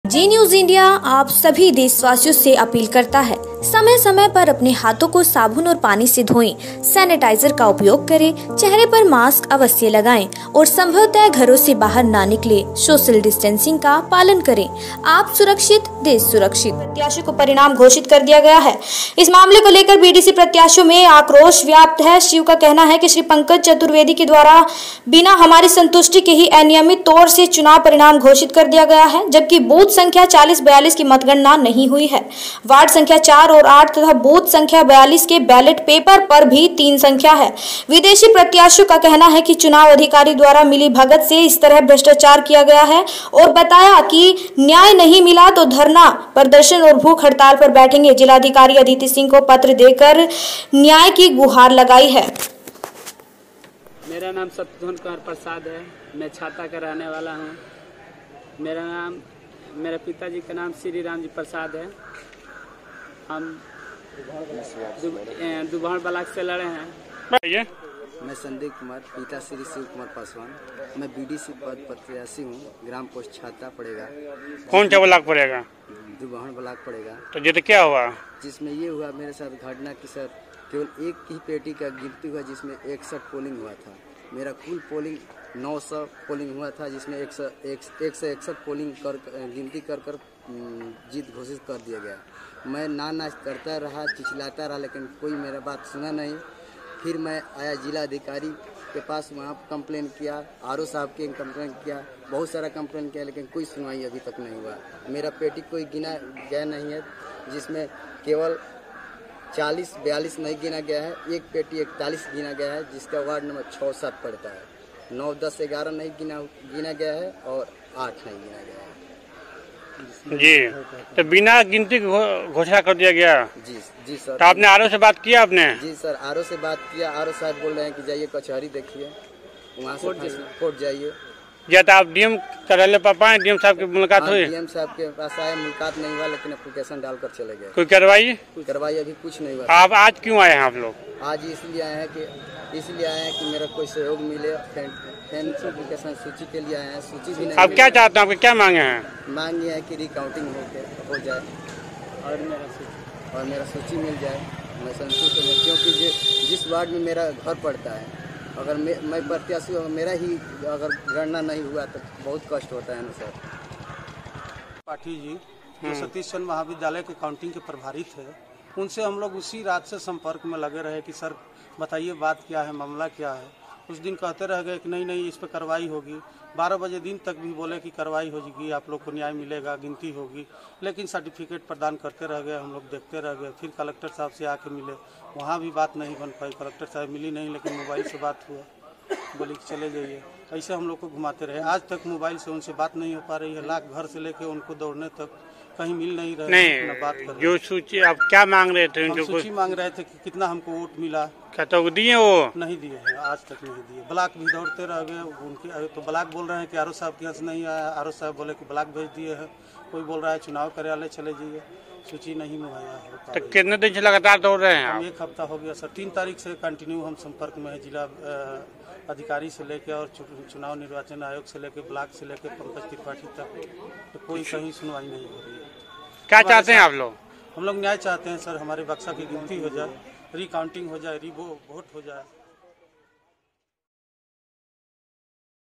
बिना मतगणना के चुनाव अधिकारी ने बीडीसी प्रत्याशीयों को परिणाम घोषित, बीडीसी प्रत्याशीयों में आक्रोश
उत्तर प्रदेश बलिया जिले में जिलाधिकारी कार्यालय पर बीडीसी प्रत्याशियों ने जिलाधिकारी को पत्र सौंपा।
दुबहड़ ब्लाक के अन्तर्गत ग्राम सभा छाता के बीडीसी प्रत्याशियों को चुनाव अधिकारी ने बिना मतगणना के ही बीडीसी प्रत्याशियों को परिणाम घोषित कर दिया गया है इस मामले को लेकर बीडीसी प्रत्याशियों में आक्रोश व्याप्त है।